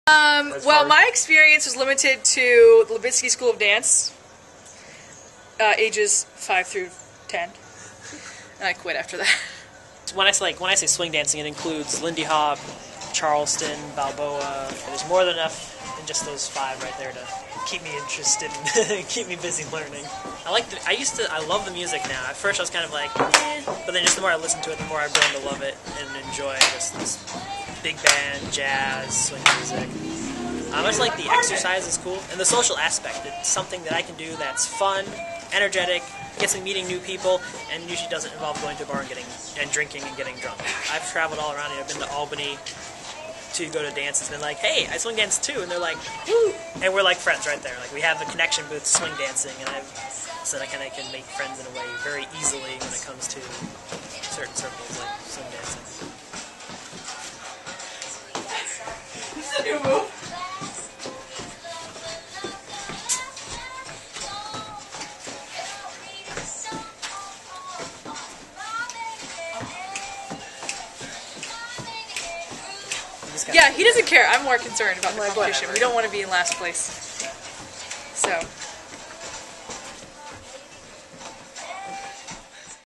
Amanda & Steven Dancing with the Ellsworth Stars
Dancing with the Ellsworth Stars contestants Amanda Ashe Look and partner Steven Kostusyki practice for Saturday, April 20 event.